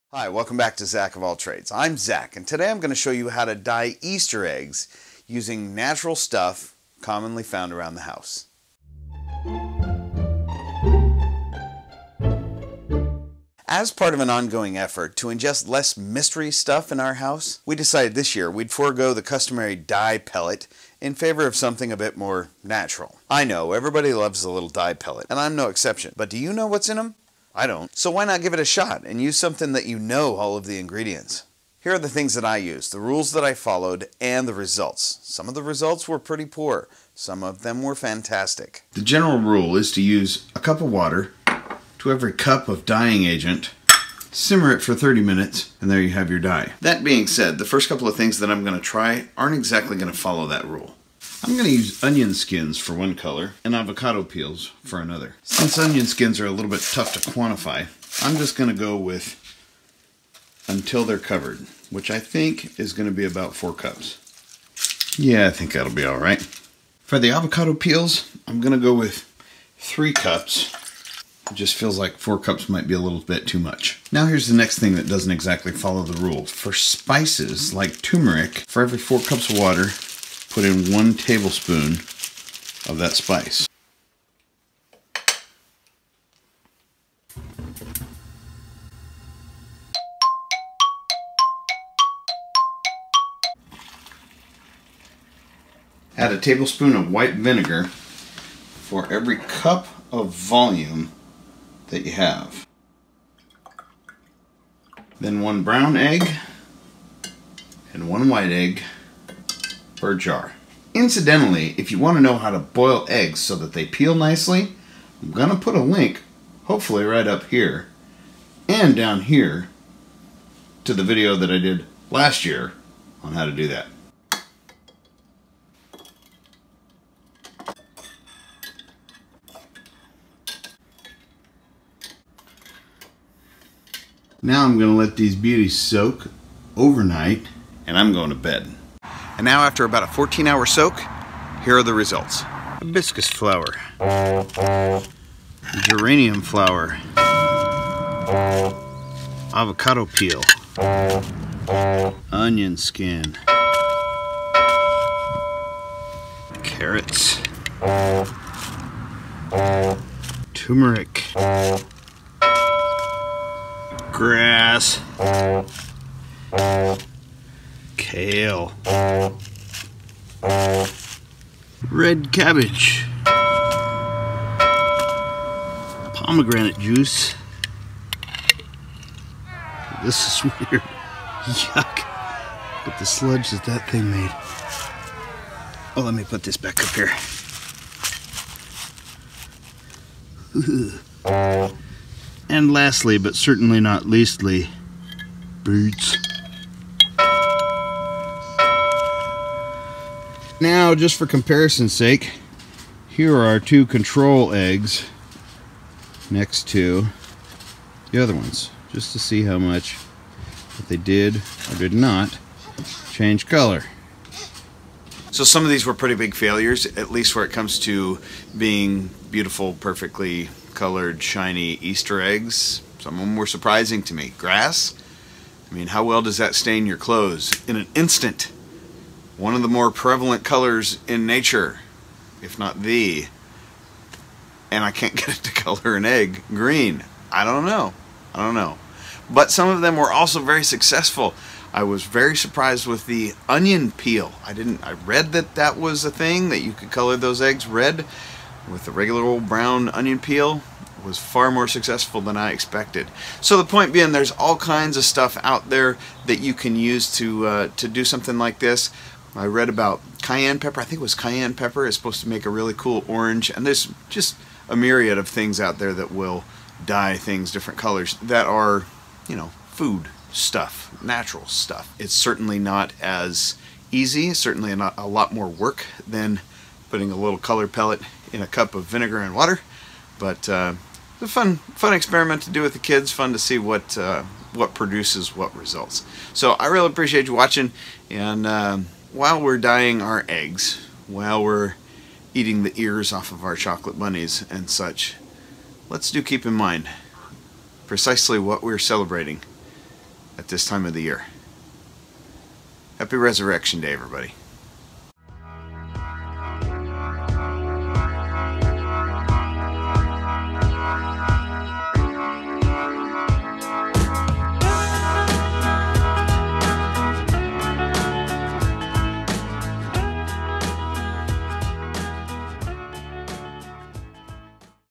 SURPRISING Homemade Easter Egg Dyes | Fun For The Whole Family
Dying easter eggs the OLD SCHOOL way, without dye pellets.